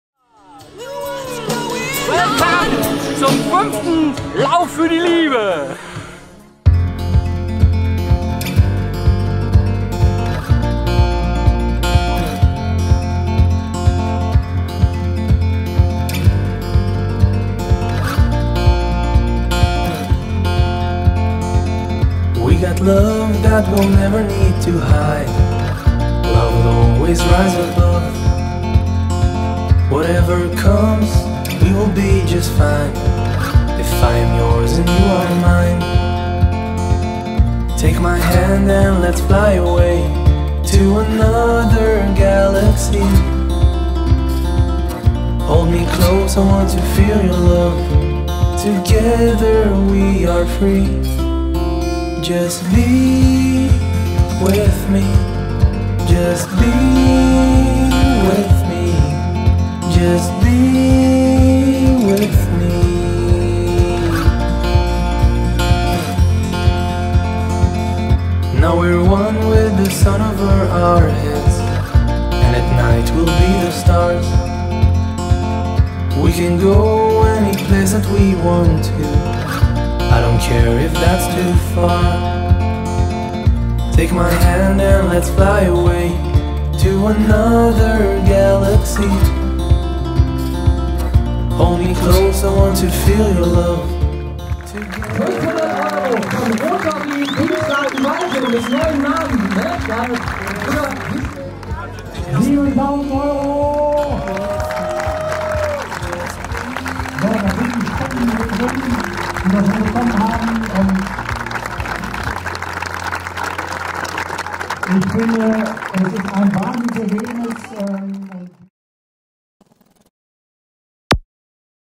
Lauf für die Liebe 2019 in Dinslaken
Der Lauf für die Liebe ist ein gemeinnüziger Lauf, der Erlös des Lauf für die Liebe wird an KidsVision gespendet. Kidsvision hat sich zur Aufgabe gemacht Familien mit behinderten Kindern zu helfen und zu unterstützen. Der Lauf wird von Roland Donner in Zusammenarbeit mit seinen vielen ehrenamtlichen Helfern und Sponsoren organisiert.
Ein Lauf für die gesamte Familie, ein Lauf für die Liebe.